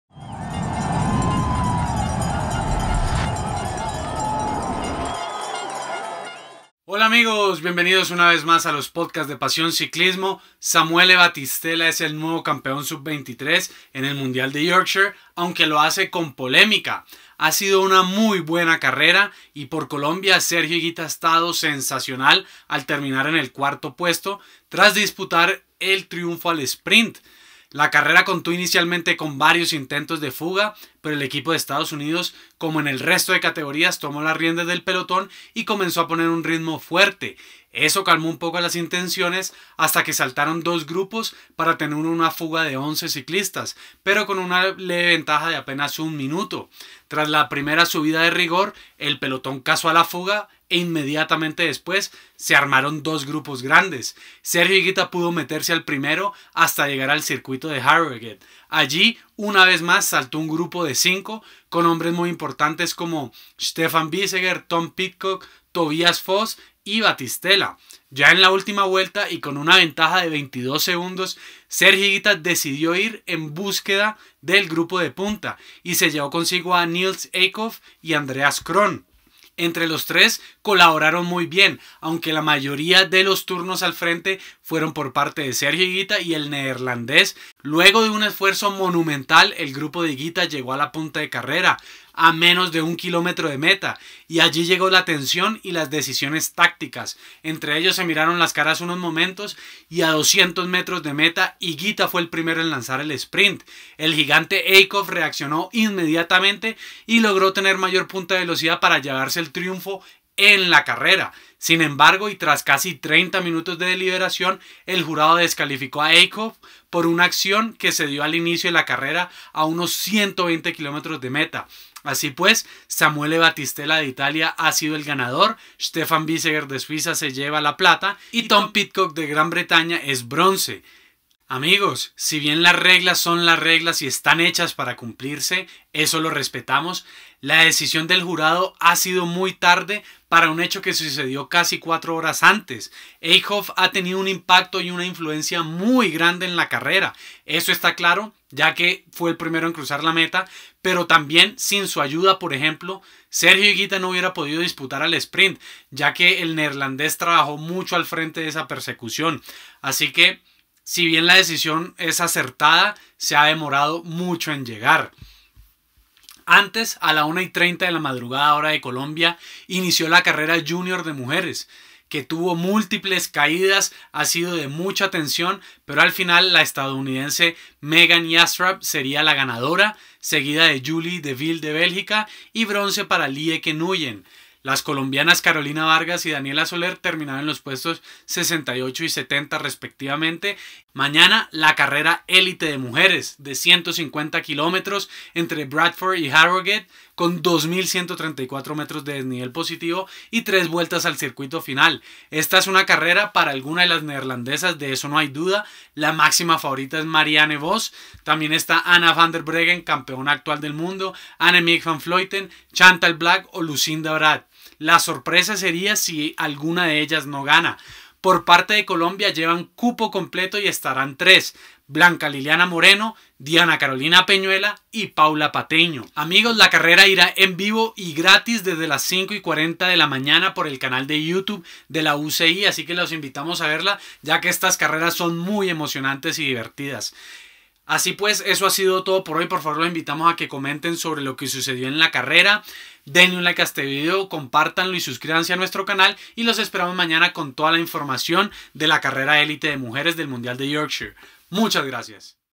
Un gran Sergio Higuita finaliza 4º en un final con polémica en Yorkshire 2019
El italiano Samuele Battistella ha ganado la prueba sub 23 del mundial de Yorkshire 2019 luego de que Nils Eekhoff haya sido descalificado. El colombiano Sergio Higuita finalizó 4º.